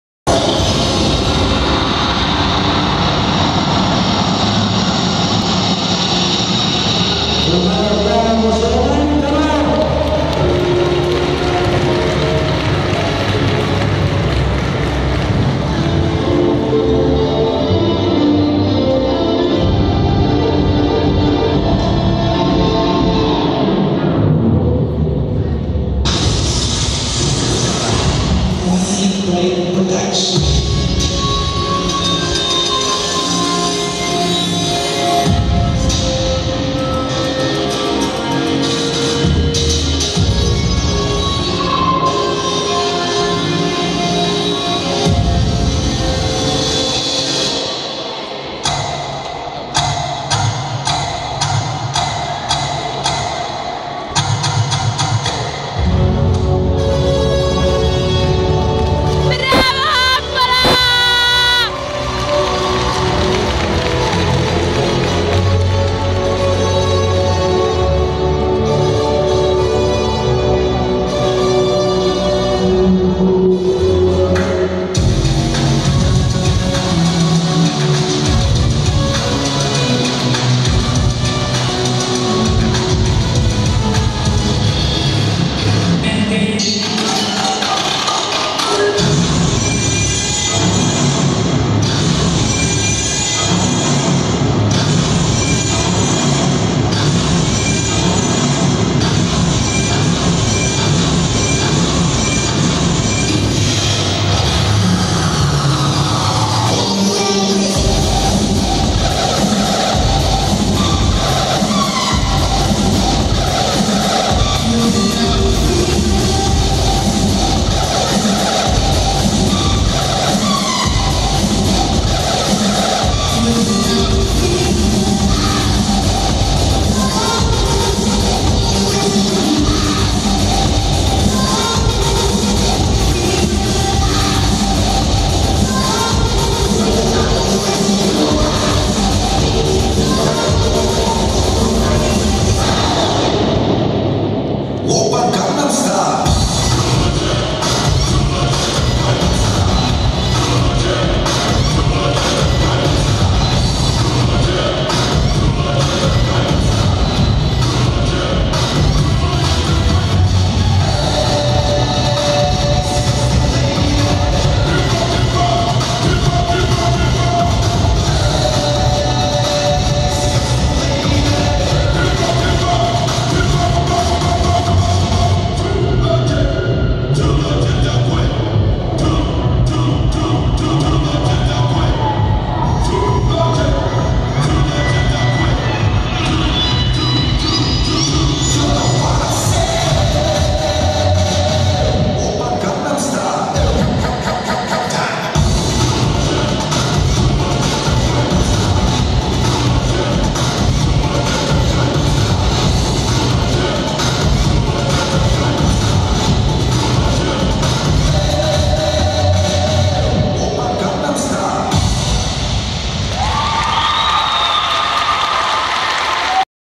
[ShowOn.it] - Electric Boogie Palasport di Calenzano (Fi)
[ Matteo Coppola - Street Dance / Hip Hop ]
Coreografia di electric boogie